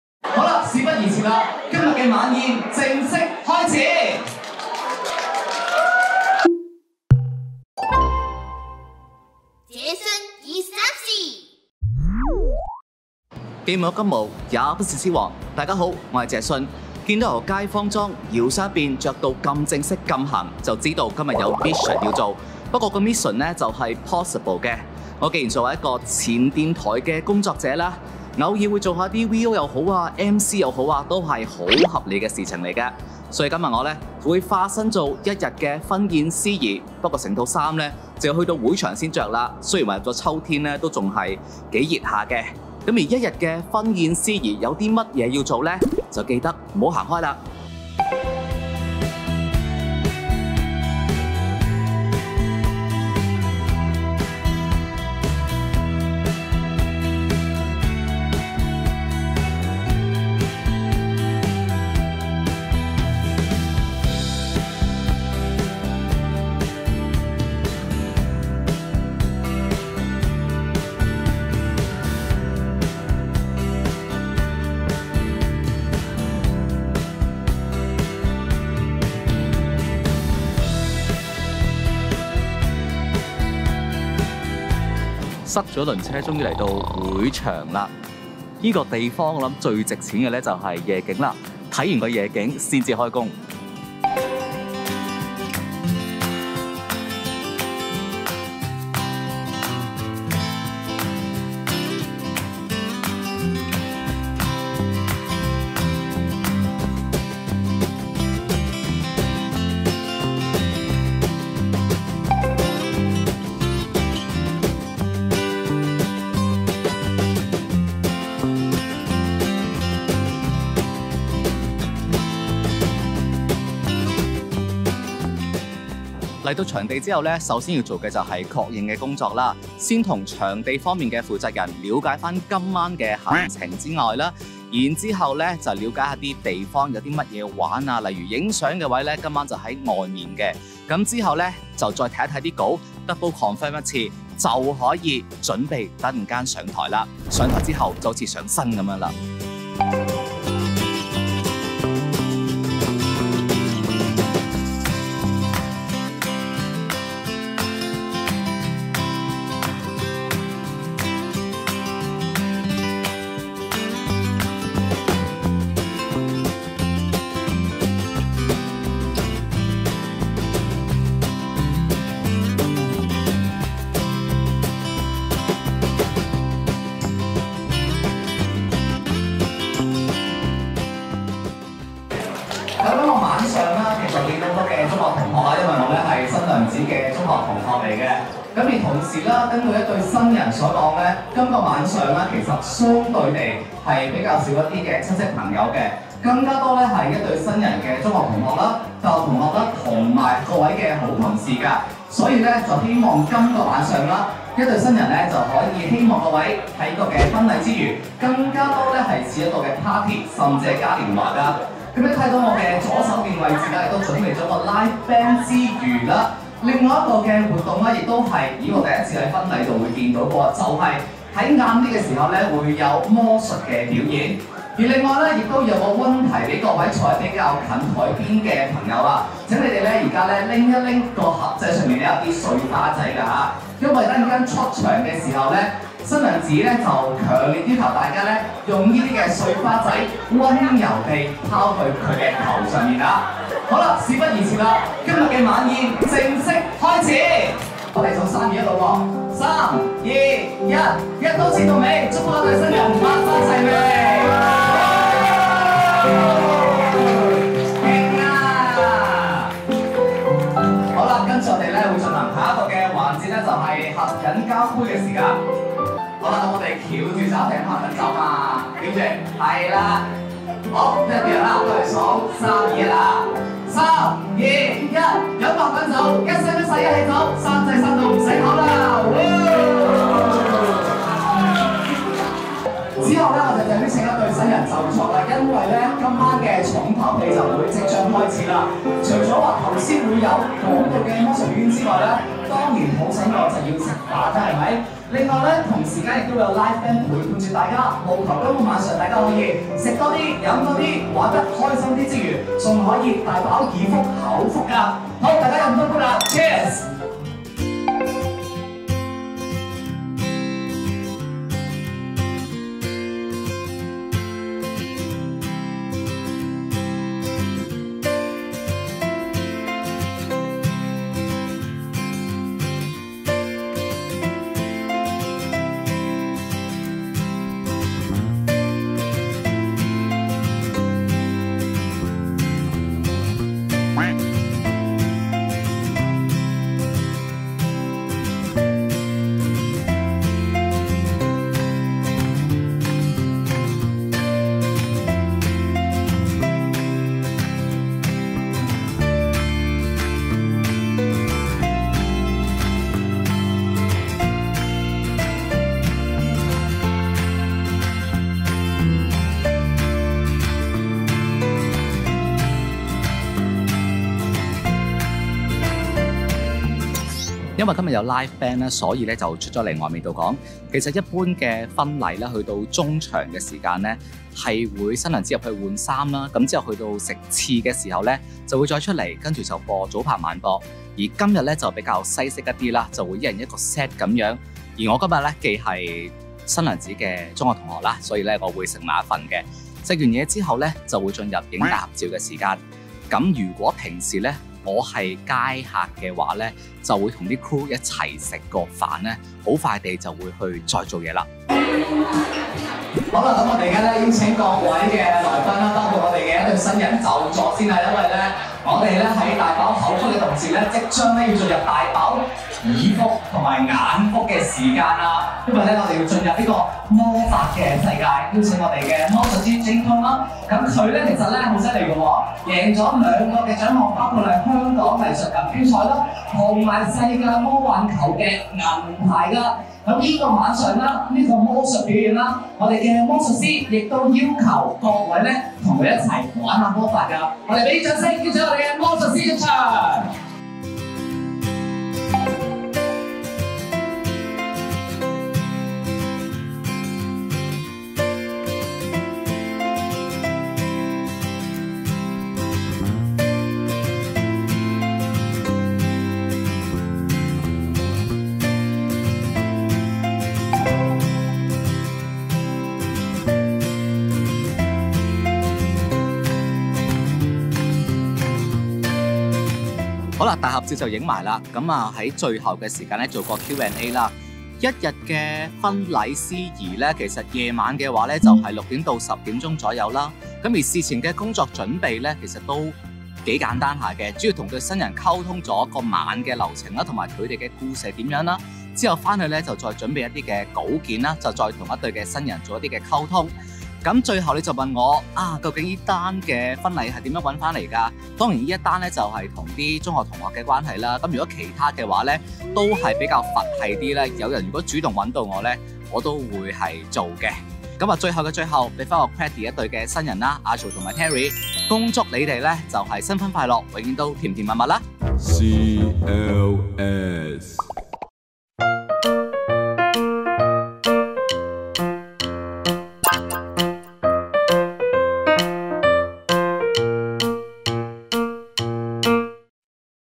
【一日MC】何謂婚宴司儀？實際是要做什麼？這一天穿上西裝跟我當一日婚宴司儀吧 ︳謝遜二三事
身為一個前電台人，偶爾會接洽司儀工作，是很合理也很合邏輯吧？這天穿上西裝，當一日婚宴司儀！

EVERYONE C.L.S.S.B. PLEASE❗❗❗